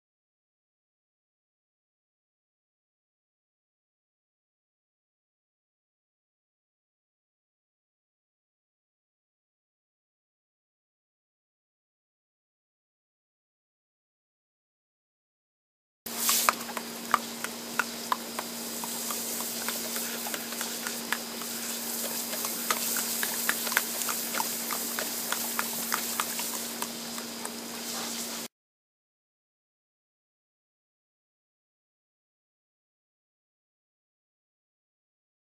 water tastes good
So I was racking my brain trying to figure out how to describe how much I appreciate all your reader (and reviewer) support for EndlessKnight.
I tried to draw something (that ended up looking like a kind of lizard, which, while interesting, was not my intention).

Then I decided to make a movie! Even though I don't know how!

E for effort?
P.S. Water was injured in the making of this video.
P.P.S. Sound on :)